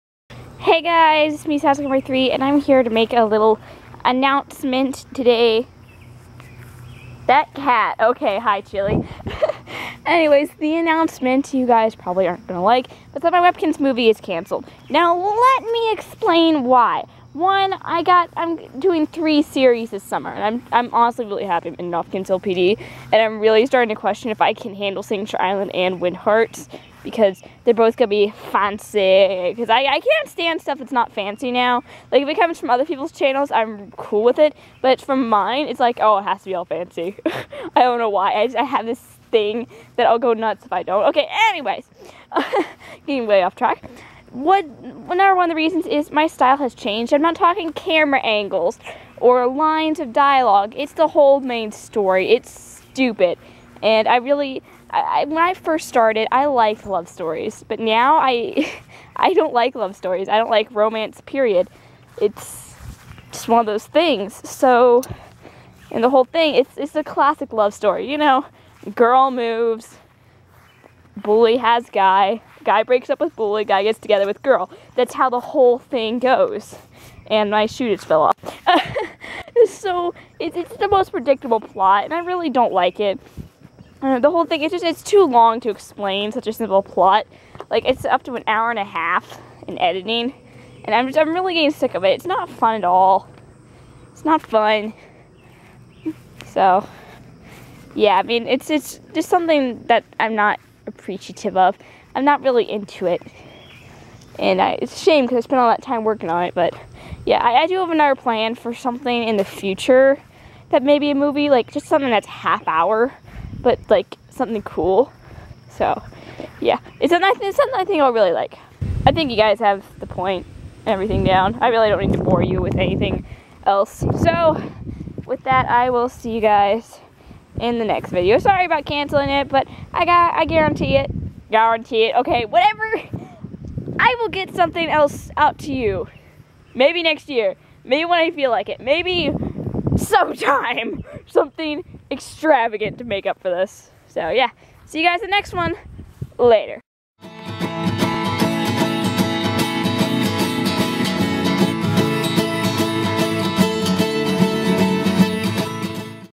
News on my Webkinz Movie
EDIT: I want to clear this up, I am NOT canceling any of my series. Wind Hearts, Signature Island and KinzVille P.D. will still go on this summer. What I am talking about is a movie I started 3 years ago and tried to pick back up last summer, not a series on my channel. Nothing of it was ever uploaded onto this channel so all anyone knew was the little information I gave out in small update videos.
Yes, I know some of you are going to be upset I canceled it, but I really think it's for the better. I wasn't happy with it at all and I hated spending an hour to get 2 minutes of footage. Plus, with KinzVille P.D., Wind Hearts AND Signature Island this summer, things are going to be rough on me. I do promise I'll make it up to you guys with something else later on.